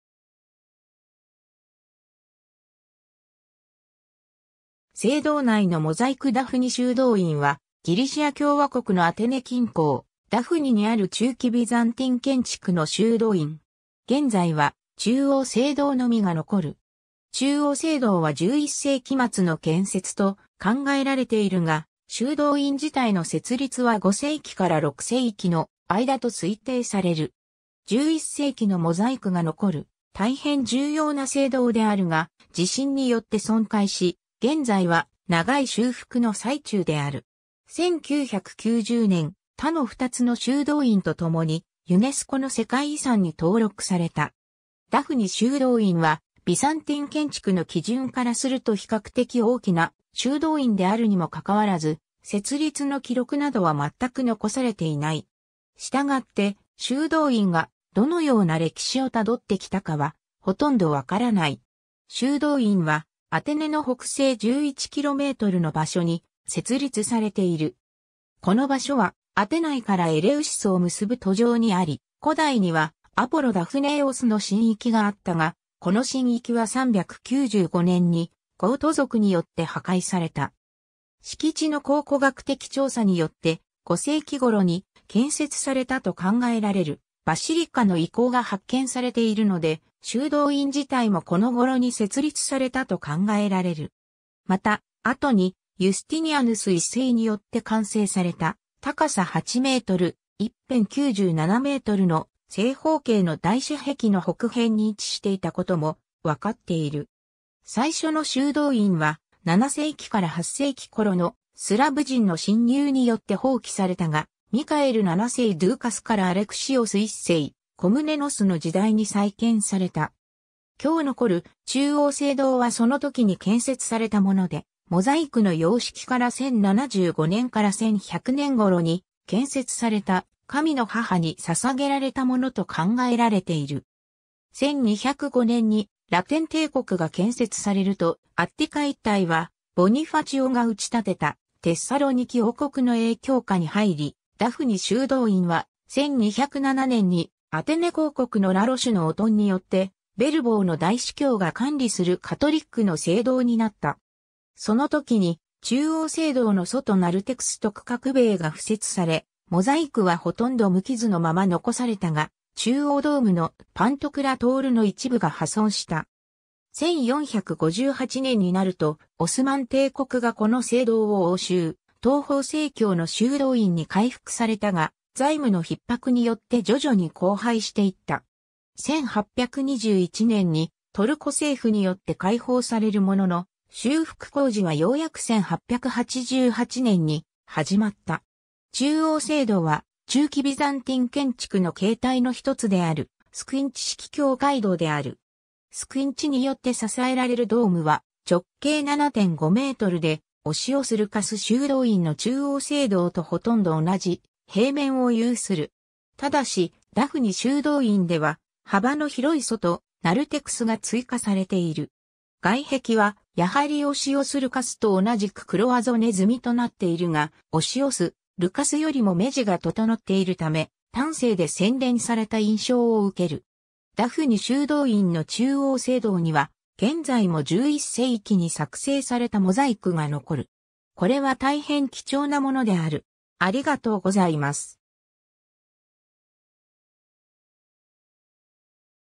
ダフニ修道院
聖堂内のモザイク ダフニ修道院（ダフニしゅうどういん、希：Μονὴ τῆς Δάφνι/Δάφνιον）は、ギリシア共和国のアテネ近郊、ダフニにある中期ビザンティン建築の修道院。
現在は中央聖堂のみが残る。
中央聖堂は11世紀末の建設と考えられているが、修道院自体の設立は5世紀から6世紀の間と推定される。
 11世紀のモザイクが残るたいへん重要な聖堂であるが、地震によって損壊し、現在は長い修復の最中である。
1990年、他の2つの修道院とともにユネスコの世界遺産に登録された（登録名は「ダフニ修道院、オシオス・ルカス修道院、ヒオス島のネア・モニ修道院」）。
 ダフニ修道院は、ビザンティン建築の基準からすると比較的大きな修道院であるにも関わらず、設立の記録などは全く残されていない。
従って、修道院がどのような歴史をたどってきたかは、ほとんどわからない。
 修道院は、アテネの北西11kmの場所に設立されている。
この場所は、アテナイからエレウシスを結ぶ途上にあり、古代にはアポロ・ダフネイオスの神域があったが、この神域は395年にゴート族によって破壊された。
敷地の考古学的調査によって、5世紀頃に建設されたと考えられるバシリカの遺構が発見されているので、修道院自体もこの頃に設立されたと考えられる。
また、後にユスティニアヌス1世によって完成された、高さ8m、一辺97mの正方形の大市壁の北辺に位置していたことも分かっている。
最初の修道院は、7世紀から8世紀頃のスラブ人の侵入によって放棄されたが、ミカエル7世ドゥーカスからアレクシオス1世コムネノスの時代に再建された。
今日残る中央聖堂はその時に建設されたもので、モザイクの様式から 1075年から1100年頃に建設された、「神の母」に捧げられたものと考えられている。
 1205年にラテン帝国が建設されると、アッティカ一帯はボニファチオが打ち立てたテッサロニキ王国の影響下に入り、ダフニ修道院は、1207年にアテネ公国のラ・ロシュのオトンによって、ベルヴォーの大司教が管理するカトリックの聖堂になった。
その時に中央聖堂の外ナルテクスと区画塀が敷設され、モザイクはほとんど無傷のまま残されたが、中央ドームの『パントクラトール』の一部が破損した。
 1458年になるとオスマン帝国がこの聖堂を押収、東方正教の修道院に回復されたが、財務の逼迫によって徐々に荒廃していった。
1821年にトルコ政府によって解放されるものの、修復工事はようやく1888年に始まった。
 中央聖堂は、中期ビザンティン建築の形態のひとつであるスクィンチ式教会堂である。
スクィンチによって支えられるドームは、直径7.5mで、オシオス・ルカス修道院の中央聖堂とほとんど同じ平面を有する。
ただし、ダフニ修道院では、幅の広い外ナルテクスが追加されている。
 外壁は、やはりオシオス・ルカスと同じくクロワゾネ積みとなっているが、オシオス・ルカスよりも目地が整っているため、端正で洗練された印象を受ける。
 ダフニ修道院の中央聖堂には、現在も11世紀に作成されたモザイクが残る。
これはたいへん貴重なものである。